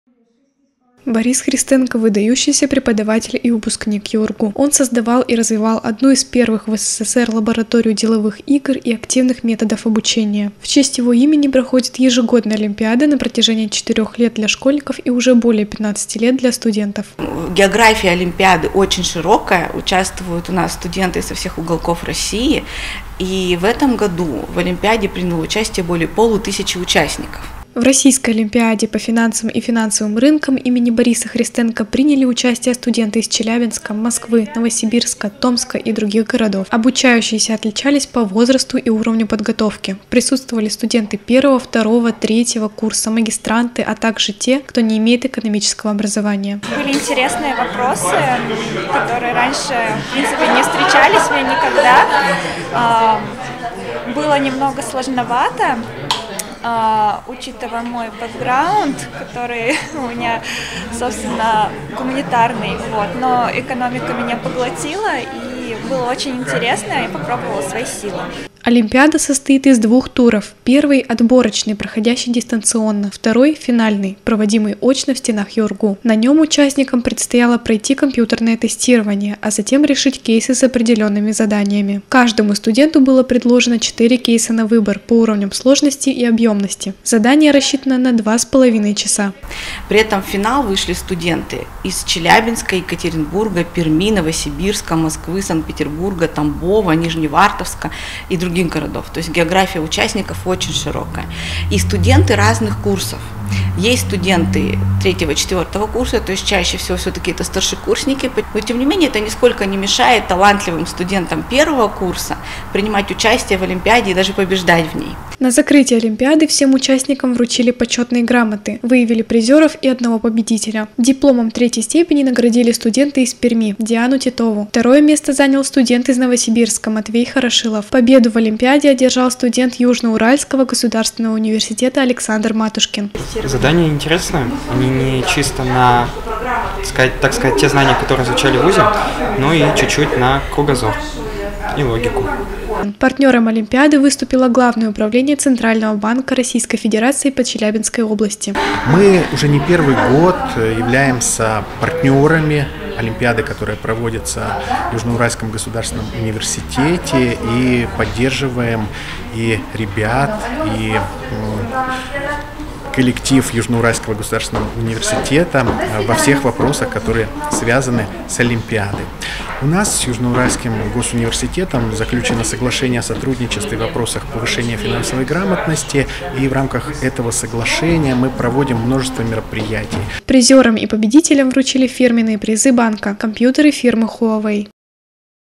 Олимпиада по финансам и финансовым рынкам
Студенты написали всероссийскую олимпиаду по финансам и финансовым рынкам имени Бориса Христенко.